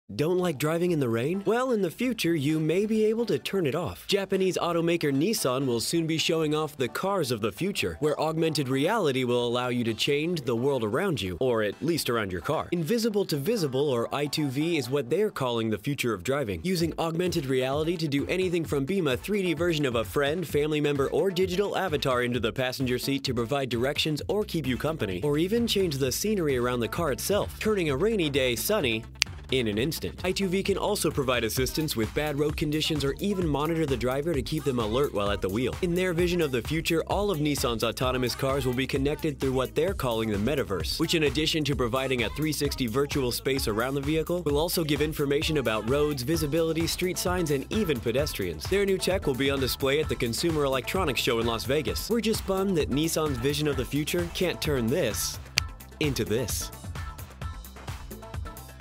'Beam' a Virtual Passenger into Your Car or 'Change' the Weather with Nissan's New Tech
Don't like driving in the rain? Nissan's I2V technology will let you turn it off. Buzz60's Tony Spitz has the details.